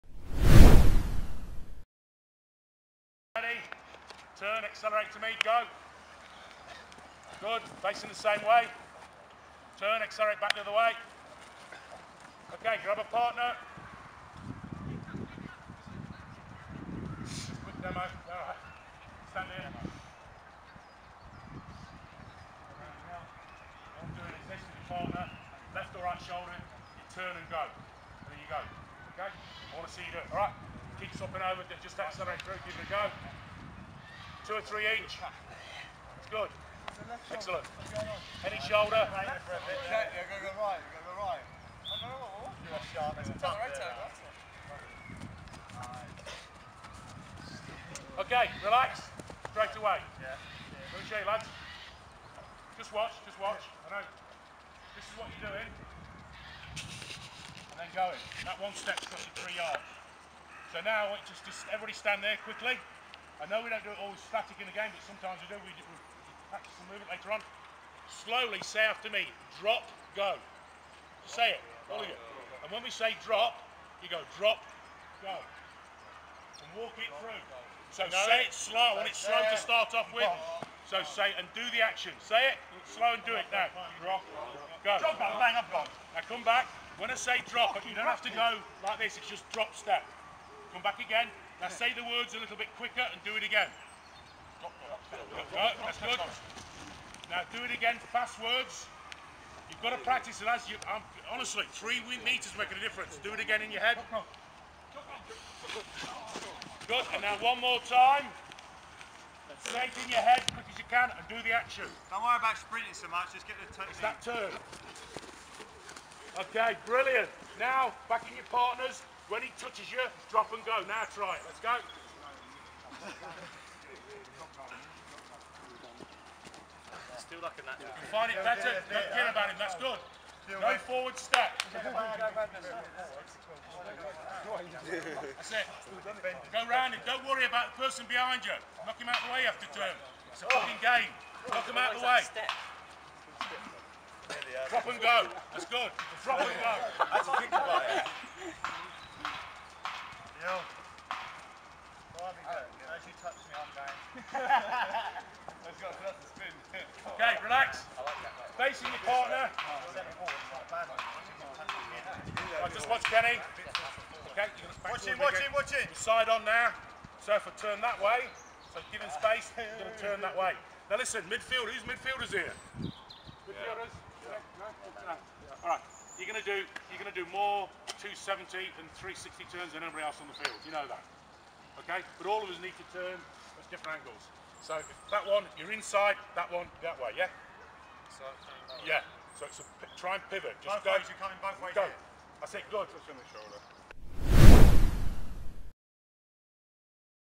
SAQ Dynamic Flex Training with Peterborough United F.C. - part 2
Alan Pearson taking a Coaching Session for SAQ Dynamic Flex Training with Peterborough United F.C. in September 2014.

In this Speed Agility and Quickness Training session during the Dynamic Flex warm up phase the players are coached with the following drills:

1.Turn and Go
2.Turn and Go - Partner Drill
3.Drop and Go
4.Drop and Go - Partner Drill